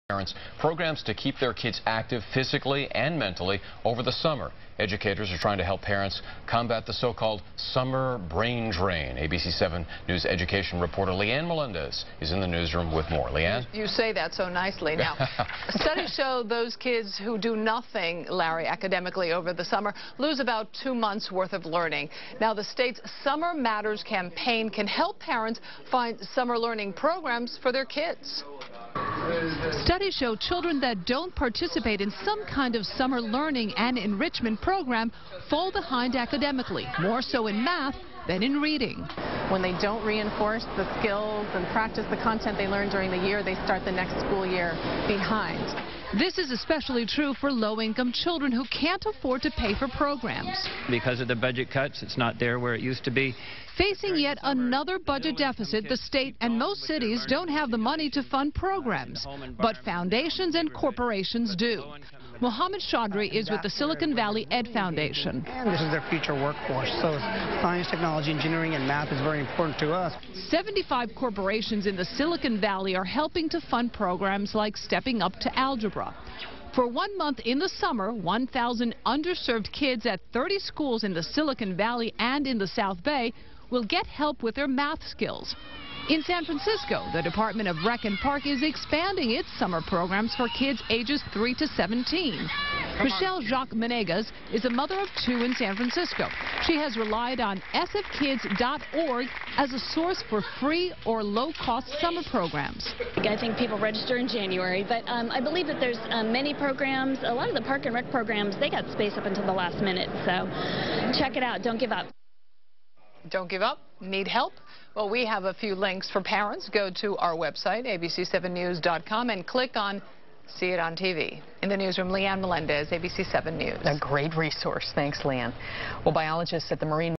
ABC news clip of kick off of statewide campaign, "Summer Matters"
SVEF helps to kick off "Summer Matters, the statewide campaign to prevent the "summer brain drain" and keep kids physically and mentally active over the summer.